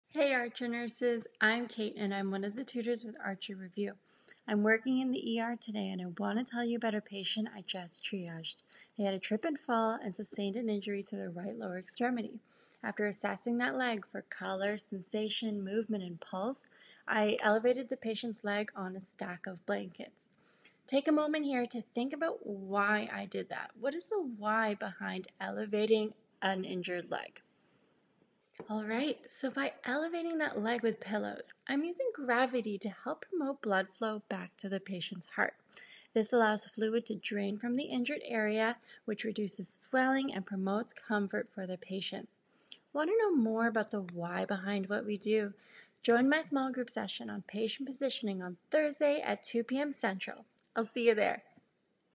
NCLEX Patient Positioning Tips
Join Cait for her small group session all about positioning and what you need to know for the Next Gen NCLEX! This one hour session will take place Thursday June 1st at 2PM CT (cost is just $25!) Here's the link to sign up ➡️